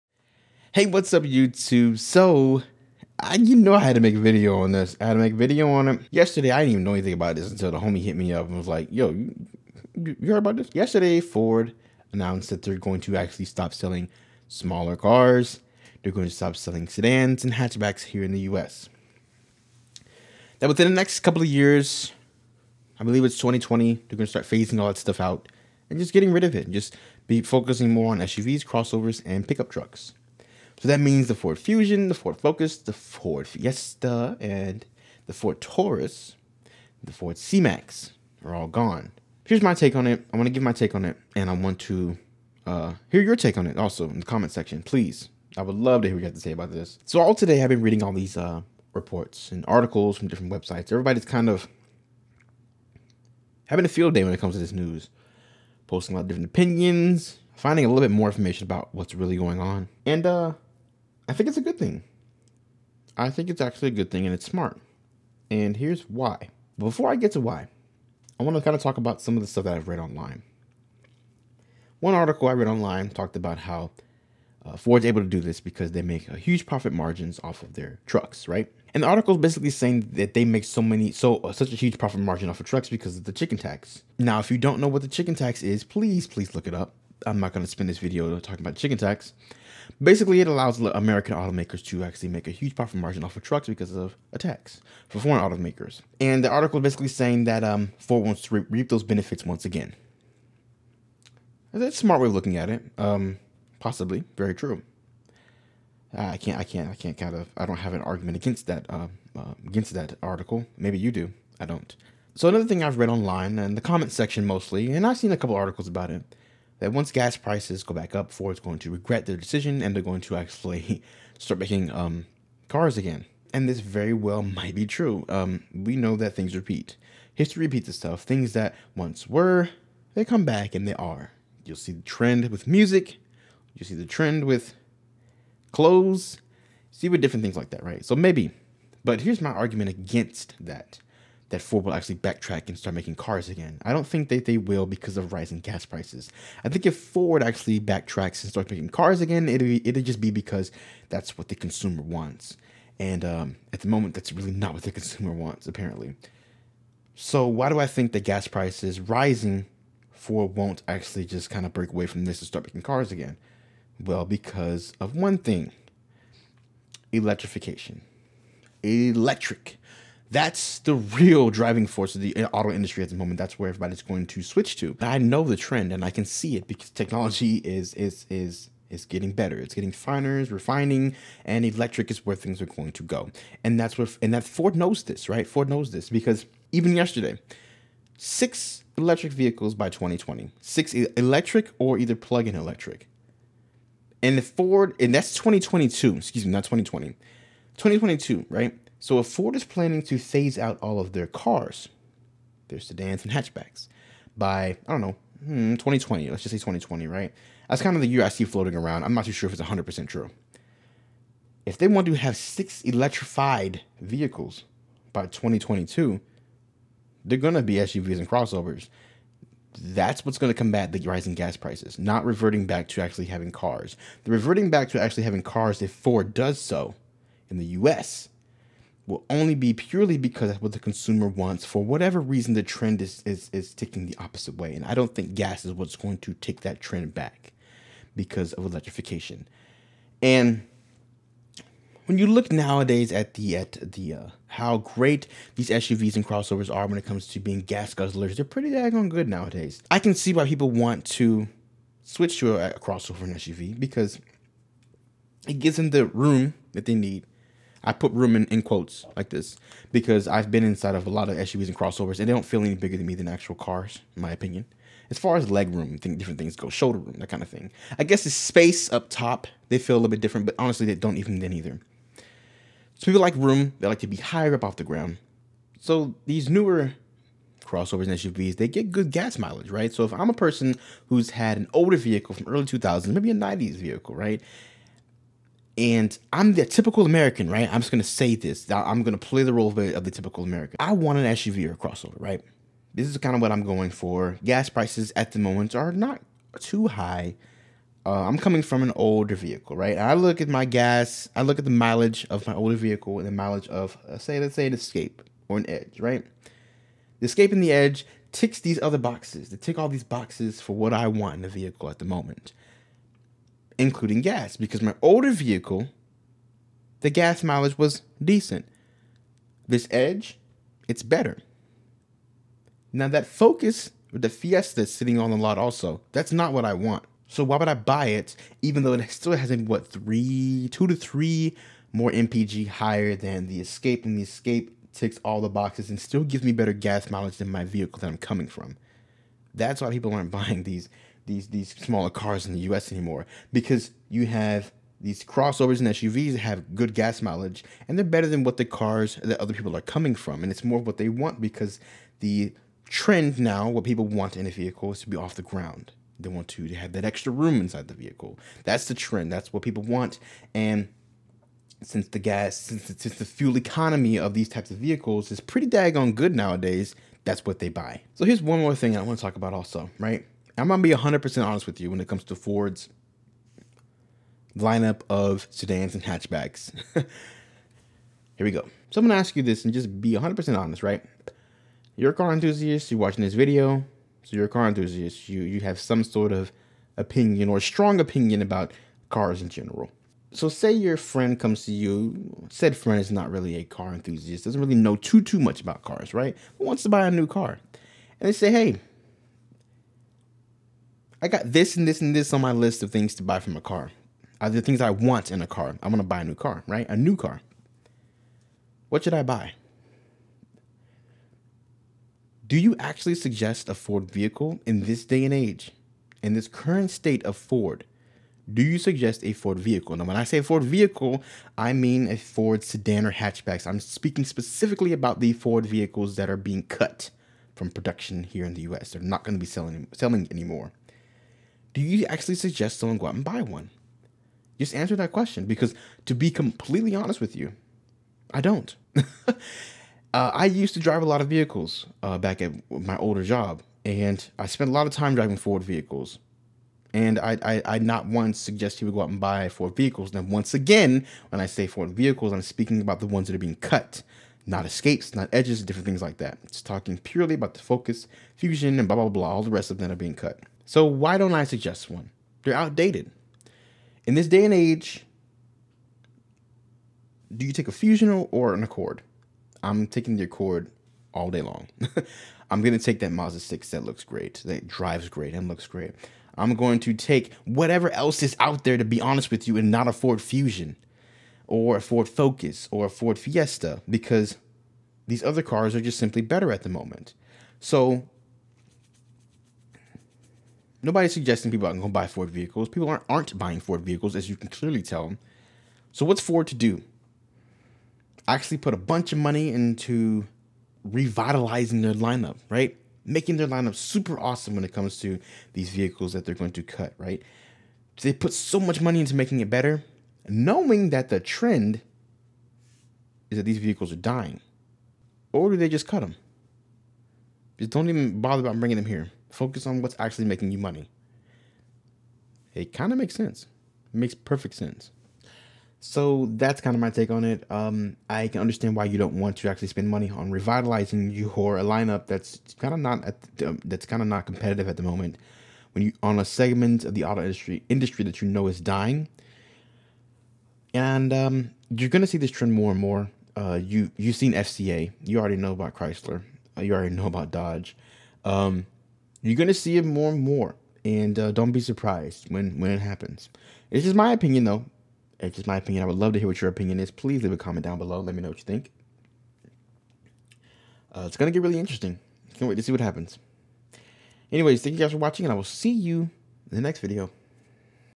Opinion: Ford Discontinuing The Fusion, Focus, Fiesta And More In America!
With the news yesterday that ford will be discontinuing all small vehicles in the North America except for the Ford Mustang and Ford Focus Active.

This is my opinion about ford no longer selling the ford fusion, focus, fiesta, taurus, and c-max in North America. I think cutting production of these small cars is devastating but makes perfect sense and here is why.